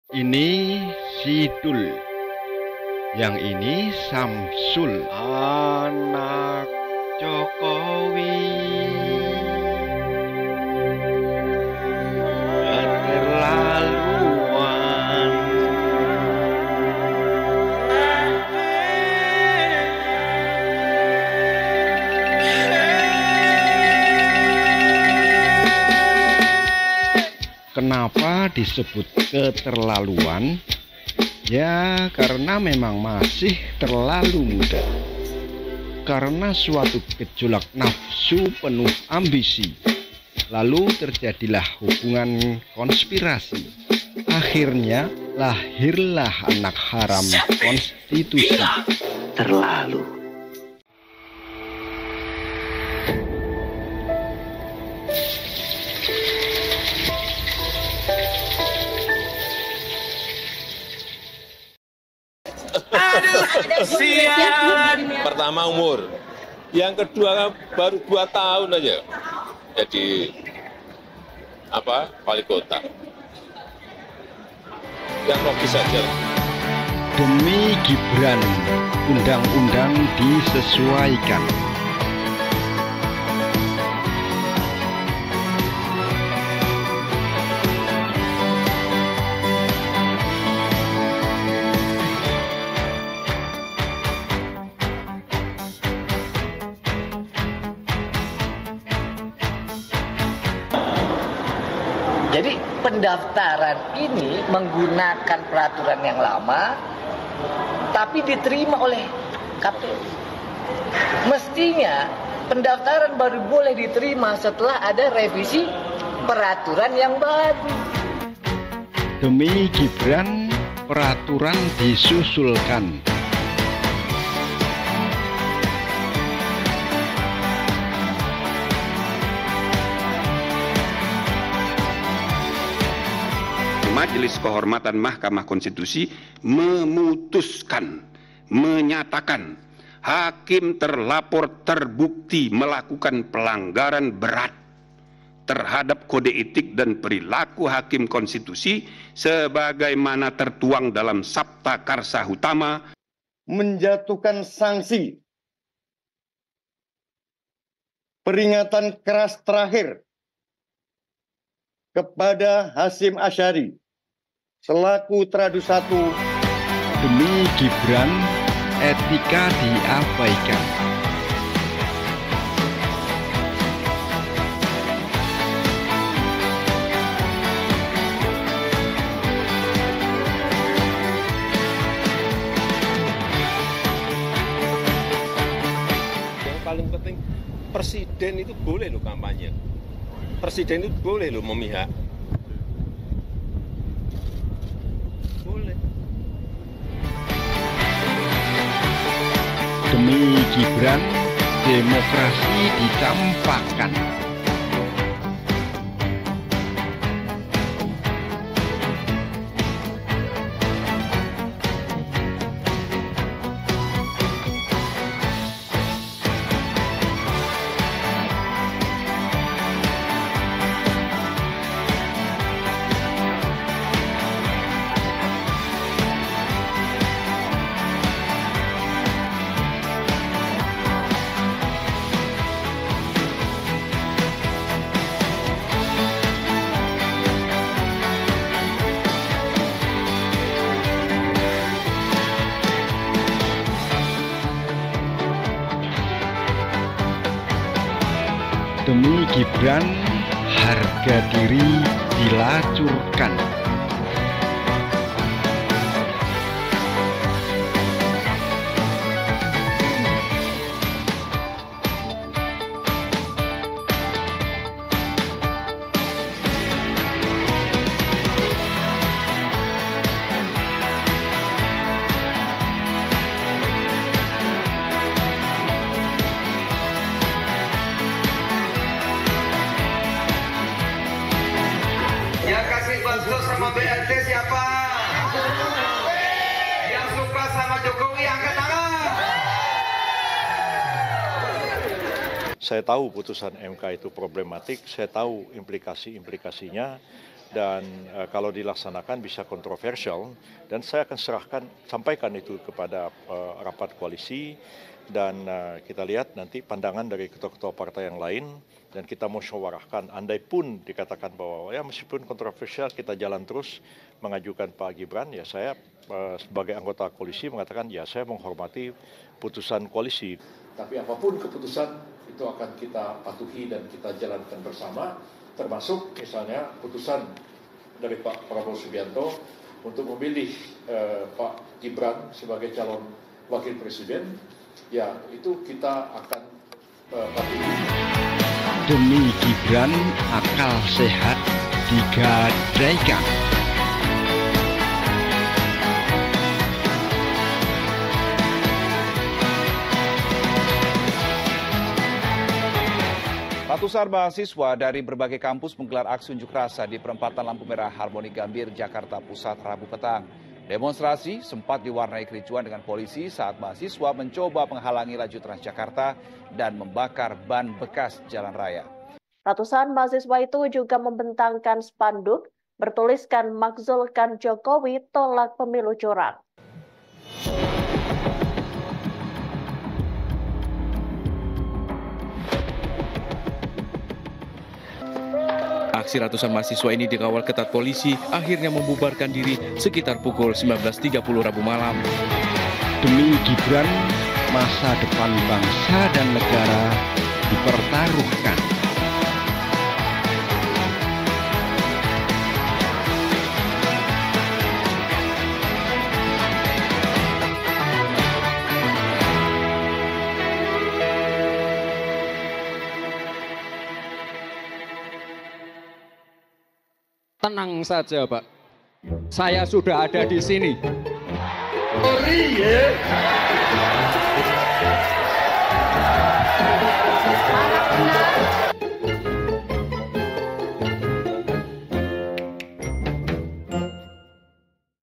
Anak Jokowi Keterlaluan.. Katenye
Anak Jokowi memang keterlaluan, disamping umurnya yang memang masih terlalu muda, prosesnya menjadi cawapres yang penuh kontroversi juga menjadikanya benar - benar keterlaluan.